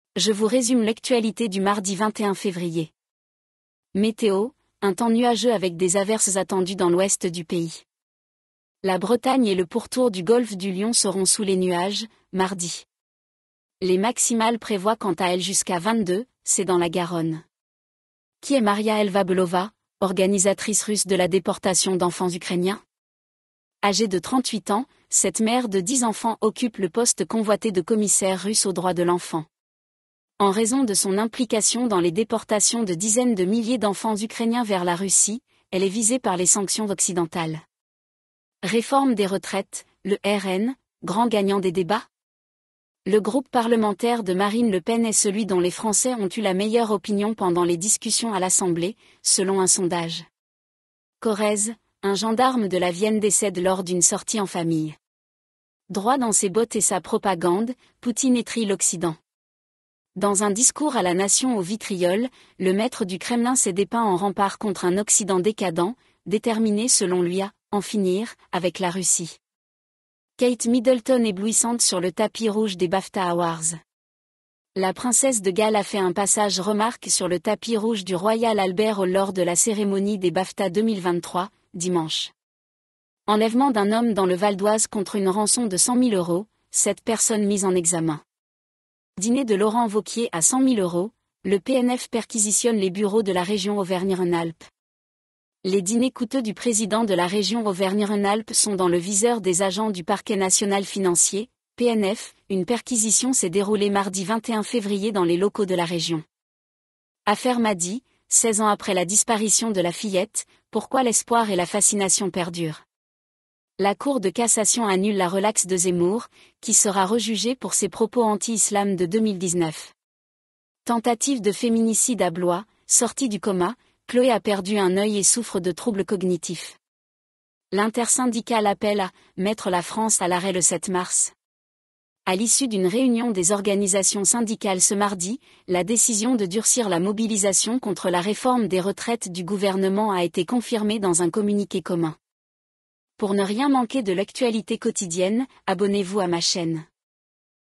Résumé de l'actualité du Mardi 21 février
- Météo : un temps nuageux avec des averses attendues dans l'ouest du pays
- Qui est Maria Lvova-Belova, organisatrice russe de la déportation d’enfants ukrainiens ?
- Réforme des retraites : le RN, grand gagnant des débats ?
- Israël : le Parlement approuve en première lecture une réforme de la justice largement contestée
- Corrèze : un gendarme de la Vienne décède lors d’une sortie en famille
- EN DIRECT - Guerre en Ukraine : Vladimir Poutine doit s’exprimer devant les députés russes, Biden prononcera un discours depuis Varsovie
- Guerre en Ukraine, en direct : Vladimir Poutine commence son discours et justifie l’invasion
- Venezuela : de faux JT présentés par des avatars prêchent la propagande de Nicolás Maduro
- « Je n’ai jamais maltraité mes enfants » : ce que l’on sait des accusations visant Poupette Kenza
- La police de Gironde recherche un héros anonyme ayant empêché le viol d’une ado de 13 ans
- Le rappeur Jul sème le chaos à Toul, une enquête ouverte
- Assassinat de Narumi : l’accusé Nicolas Zepeda récuse son avocat, le procès en appel renvoyé à jeudi
- Que chantera La Zarra, la chanteuse québécoise qui représentera la France à l’Eurovision ?
- Droit dans ses bottes et sa propagande, Poutine étrille l’Occident
- Kate Middleton éblouissante sur le tapis rouge des Bafta Awards
- Enlèvement d’un homme dans le Val-d’Oise contre une rançon de 100 000 euros : 7 personnes mises en examen
- États-Unis : après la tuerie à l’université du Michigan, les étudiants réclament l’encadrement des armes
- Pyrénées-Orientales : sans permis de conduire et accompagné de son fils de 10 ans, il tente de passer 9 900 litres d’alcool en contrebande
- 3-MMC, Buddha Blues… pourquoi les drogues de synthèse posent de nombreux problèmes aux autorités
- Dîner mondain de Laurent Wauquiez : perquisition du PNF à la région Auvergne-Rhône-Alpes
- La théorie du «boyfriend air» qui se répand sur TikTok, c’est quoi ?
- Pierre Palmade soupçonné de pédopornographie : son état de santé incompatible avec une garde à vue
- Dîners de Laurent Wauquiez à 100 000 euros : le PNF perquisitionne les bureaux de la région Auvergne-Rhône-Alpes
- Affaire Maddie : 16 ans après la disparition de la fillette, pourquoi l’espoir et la fascination perdurent
- La Cour de cassation annule la relaxe de Zemmour, qui sera rejugé pour ses propos anti-Islam de 2019
- Dunkerque : un migrant tué par balle, une information judiciaire pour homicide ouverte
- Berlusconi, l’encombrant et irremplaçable allié de Meloni
- Silence on joue ! Spécial événement «Furax» en stream contre les violences sexistes et sexuelles, avec Nat’Ali et Joul
- L’obésité en forte progression chez les jeunes adultes français
- Homme mortellement poignardé en pleine rue à Villiers-sur-Marne : le suspect activement recherché
- Affaire Jubillar : deux Peugeot focalisent l’attention de la justice, un chassé-croisé entre voitures suspectes
- L’UE veut interdire le chalutage de fond dans les aires marines protégées
- Canaux vides à Venise, Garonne à sec à Toulouse, lac au plus bas en Autriche: les images d’une sécheresse hivernale historique
- De l’éther « pour éviter qu’elle souffre » : la glaçante confrontation entre Dominique P. et sa victime
- Deux militants d’extrême droite condamnés pour des tweets visant le recteur de la mosquée de Lyon
- Affaire Solère : les demandes de nullité de la procédure devront être réexaminées par la justice
- Incendie mortel à Lyon : sans sa compagne et sa fille, Julien « essaie tant bien que mal de survivre »
- Macron loue la «valeur travail» à Rungis, duel à distance Poutine-Biden, la France «à l’arrêt» le 7 mars… l’actu de ce mardi 21 février
- Biden et Poutine : duel à distance
- Tentative de féminicide à Blois : sortie du coma, Chloé a perdu un œil et souffre de troubles cognitifs
- L’intersyndicale appelle à «mettre la France à l’arrêt le 7 mars»
- Brésil : les recherches se poursuivent après les intempéries, déjà 44 morts
- Darmanin traité de « violeur » : un homme sera rejugé pour outrage
- Le ministre de la Justice annonce une évolution « historique » du statut des agents pénitentiaires
- «Last Dance», drag à l’âme
- Le corps d'un étudiant disparu en 1976 retrouvé au fond d'un ruisseau
- Au Royaume-Uni, la semaine de quatre jours rencontre un franc succès